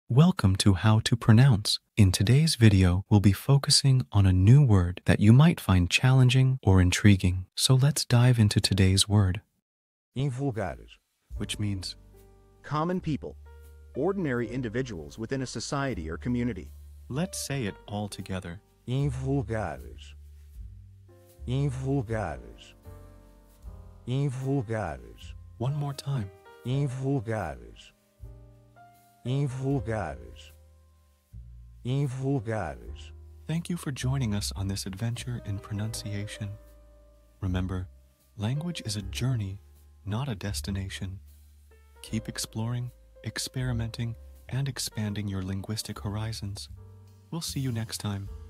How to pronounce Invulgares
Master the Pronunciation of 'Invulgares UNIVERSAL WHICH' - which means : Common People;Ordinary Individuals Within the Society or Community. 📝 with @HTPronounce 🎓🗣️ - [Your Guide to Portuguese]

Learn to pronounce 'Invulgares' perfectly in Portuguese with our easy and engaging tutorial by @HTPronounce! 📖📽️ In this comprehensive video, we provide you with native speaker examples 🗣️👂 and phonetic breakdowns 🎵 to ensure you get the pronunciation just right.

How to say Uninvial in Portuguese